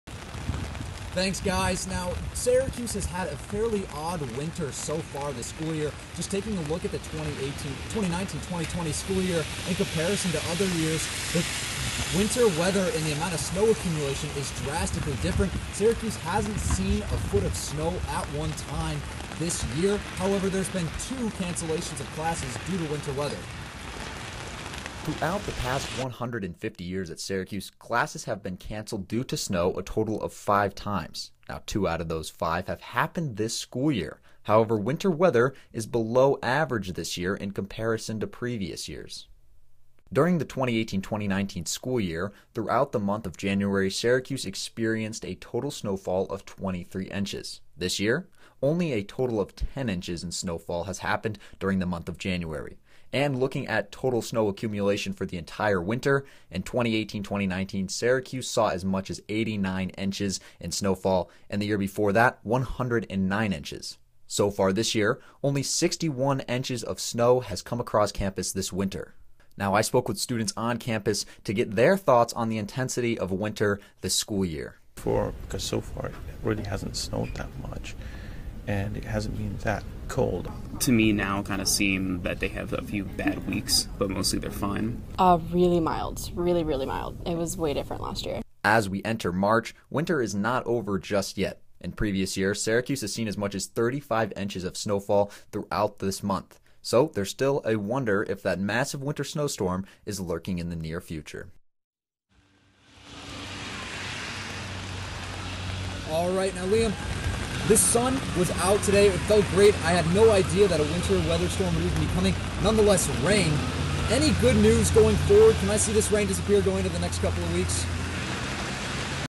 Live at 6 | Syracuse Winter Weather
Reporter Walker Simmons is at Crouse-Hinds to tell us about the snow that Syracuse has been getting this winter.